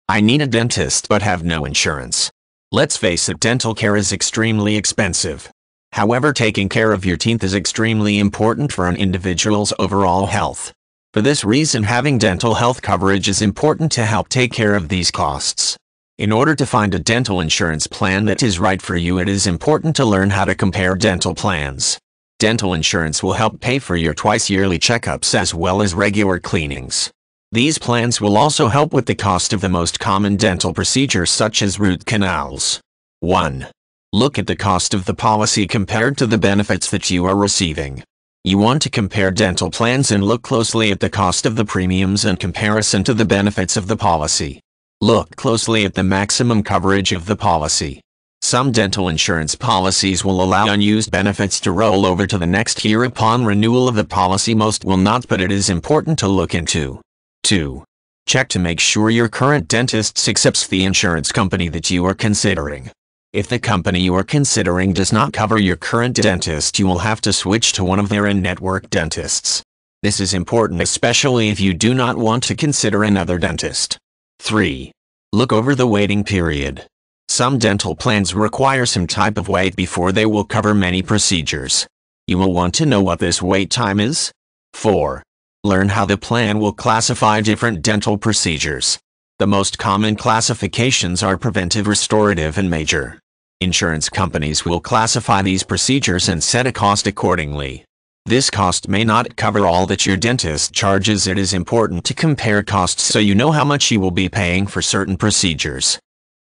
I Need A Dentist But Have No Insurance
In today's economy, the traditional dental insurance model can be cost prohibitive to businesses, causing them to eliminate dental care from their benefit package. Unfortunately, paying the full cost for dental procedures without any insurance is expensive and cost prohibitive as well. Many people make a decision to forgo dental care or wait until there is an emergent, expensive dental crisis before seeking treatment. However, there is another alternative. Affordable dental insurance plans help reduce the cost of dental care by ten to sixty percent, provide access to a large network of participating dentists, includes such services as root canals, crowns and braces (to name only a few), as well as include discounted dental products. There are not any exclusion clauses for prior dental health conditions and these low cost plans provide excellent individualized customer service.